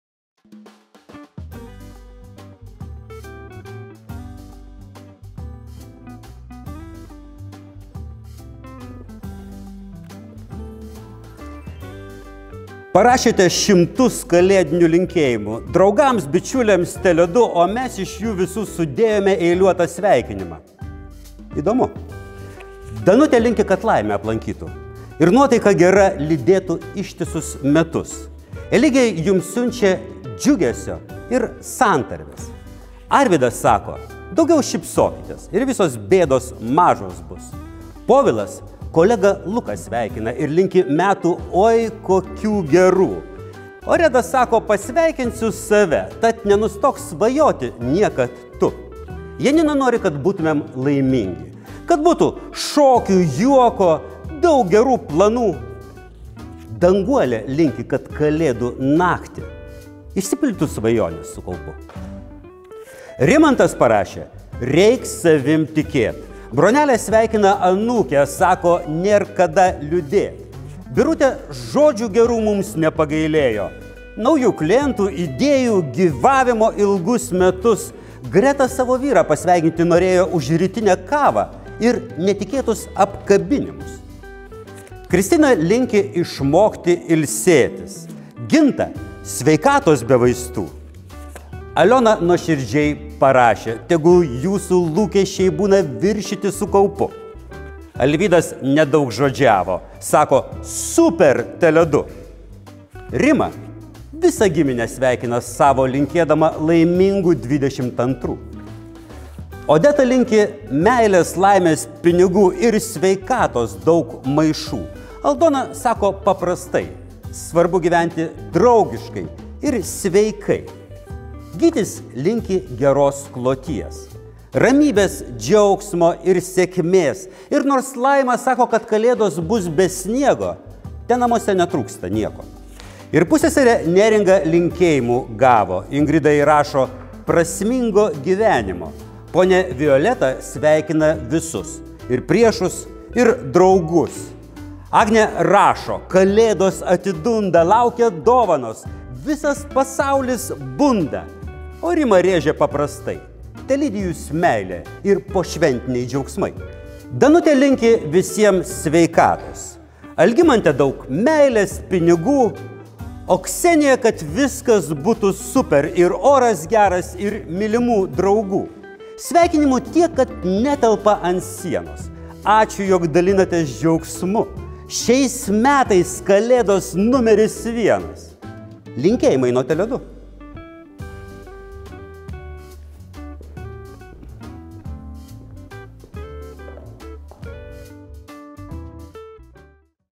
TELE2 kalėdinė sveikinimų poema nr. 1!
Sudėjome jūsų kalėdinius sveikinimus į vieną vietą ir taip gimė TELE2 kalėdinė sveikinimų poema nr. 1! Linkime visko: svajoti, bendrauti, šėlti, nebijoti ir visada keliauti aukštai iškėlus galvą. Juk šios Kalėdos yra numeris VIENAS! Linksmų ir nepamirštamų švenčių! Linkėjimai nuo TELE2!